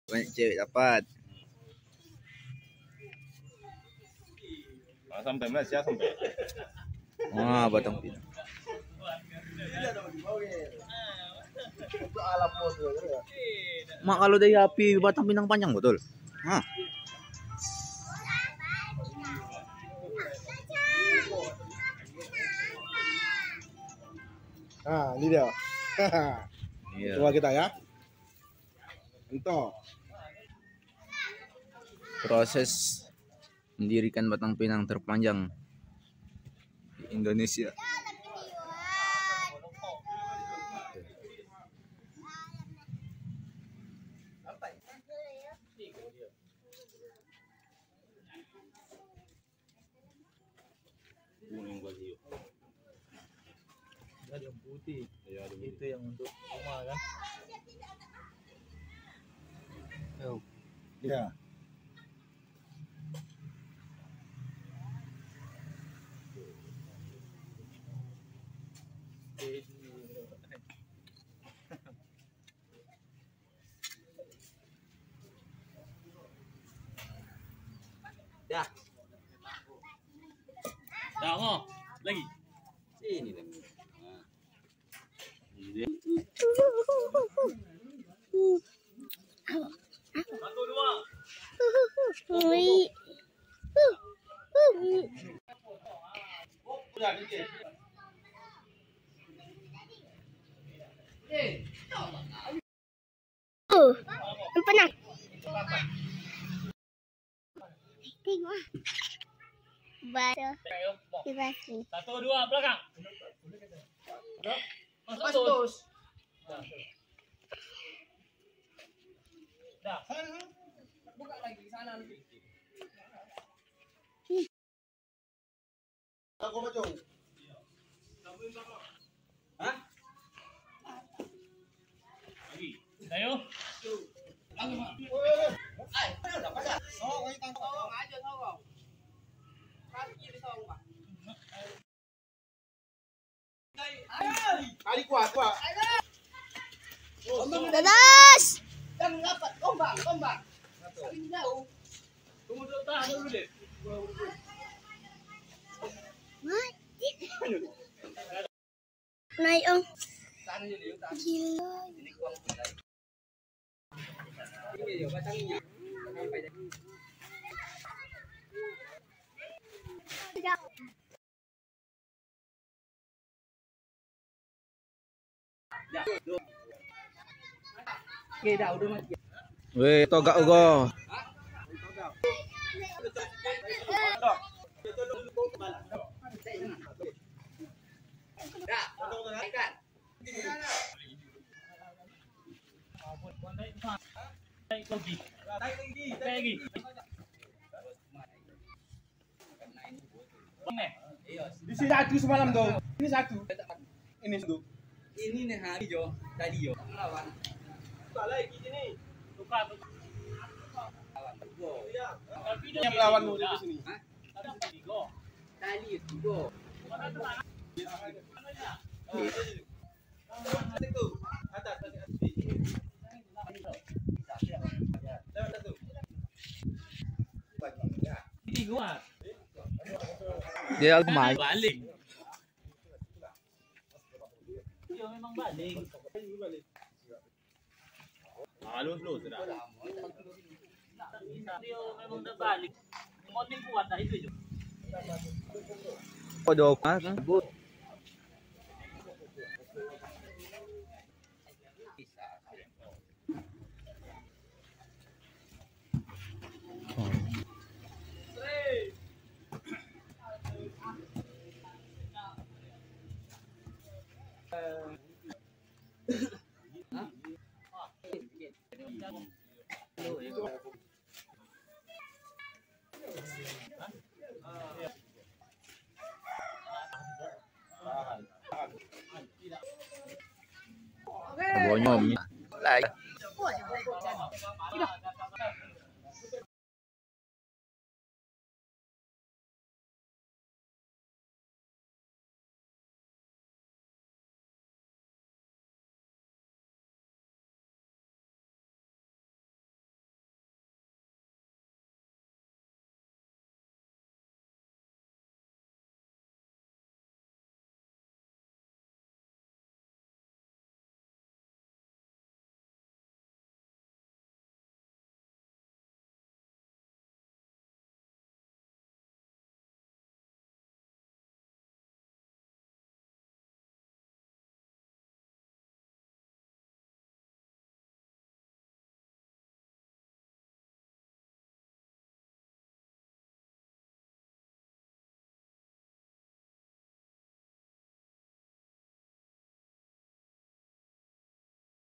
begini la proses mendirikan batang pinang terpanjang di indonesia 12 meter
proses mendrikan batang pinang